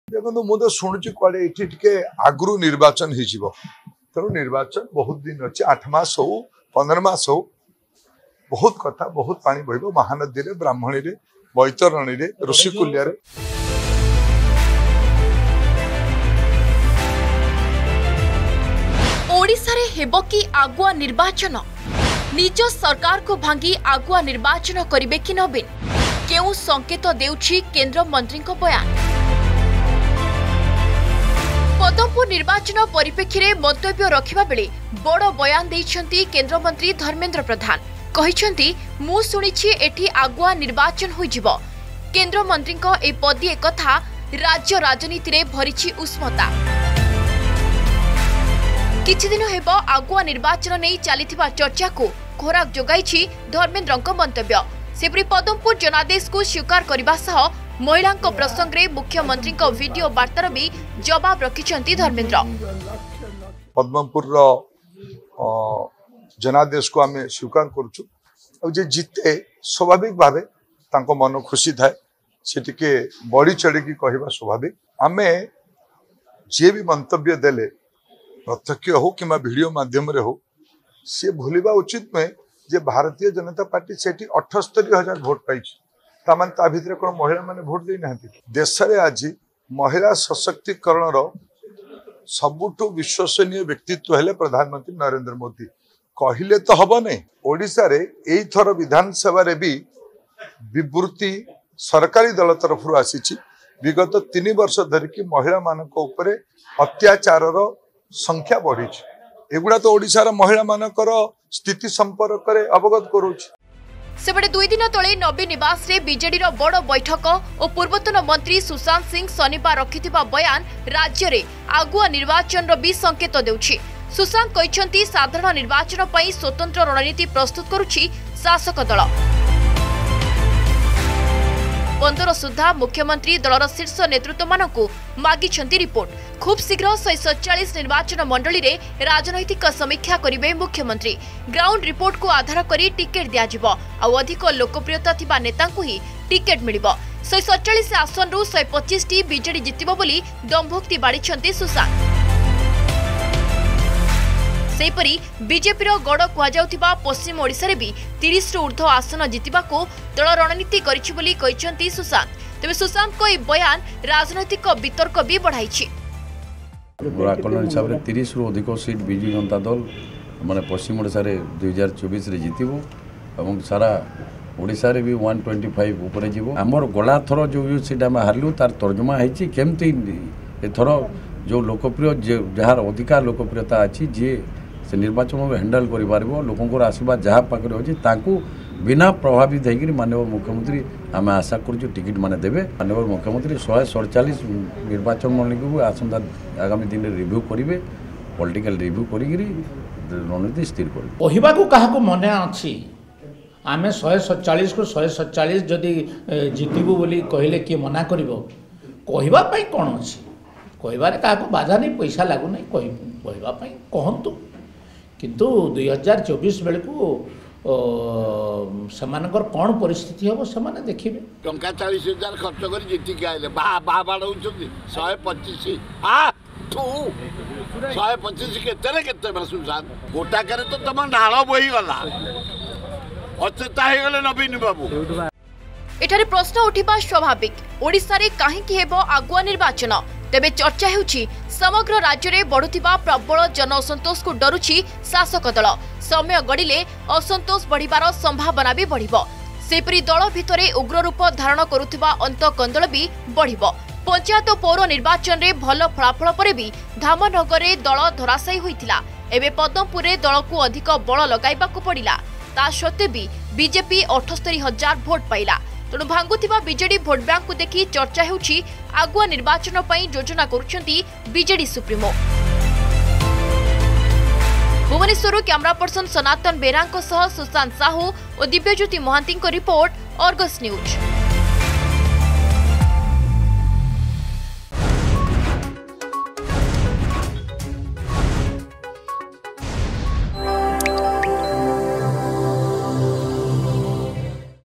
ଓଡିଶାରେ ହେବ କି ଆଗୁଆ ନିର୍ବାଚନ ?
ନିଜ ସରକାରକୁ ଭାଙ୍ଗି ଆଗୁଆ ନିର୍ବାଚନ କରିବେ କି ନବୀନ  ?
ପଦମପୁର ନିର୍ବାଚନ ପରିପ୍ରେକ୍ଷୀରେ ମନ୍ତବ୍ୟ ରଖିବା ବେଳେ ବଡ ବୟାନ ଦେଇଛନ୍ତି କେନ୍ଦ୍ର ମନ୍ତ୍ରୀ ଧର୍ମେନ୍ଦ୍ର ପ୍ରଧାନ । କହିଛନ୍ତି "ମୁଁ ଶୁଣୁଛି ଏଠି ଆଗୁଆ ନିର୍ବାଚନ ହୋଇଯିବ" । କେନ୍ଦ୍ର ମନ୍ତ୍ରୀଙ୍କ ଏହି ପଦିଏ କଥା ରାଜ୍ୟ ରାଜନୀତିରେ ଭରିଛି ଉଷ୍ମତା ।